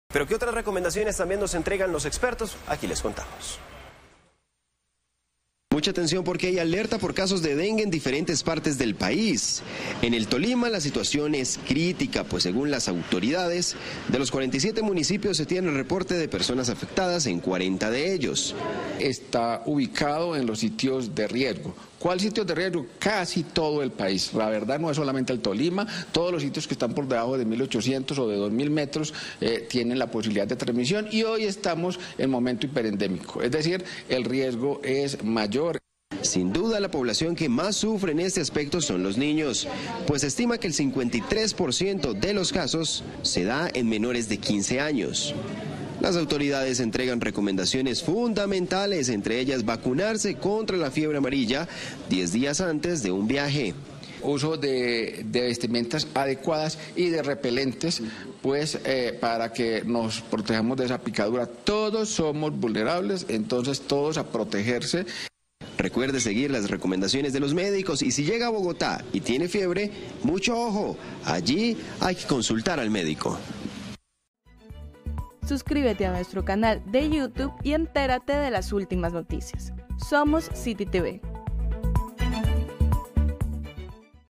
Los casos de dengue se incrementaron en Colombia | CityTv
En departamentos como el Tolima la situación es crítica, pues según las autoridades, de los 47 municipios del departamento se tienen reportes de personas afectadas en 40 de ellos. Las autoridades consideran que se atraviesa una situación de riesgo hiperendémico. El 53% de los casos se dan en menores de 15 años, ante esta situación, las autoridades entregan recomendaciones. Todos los detalles a continuación.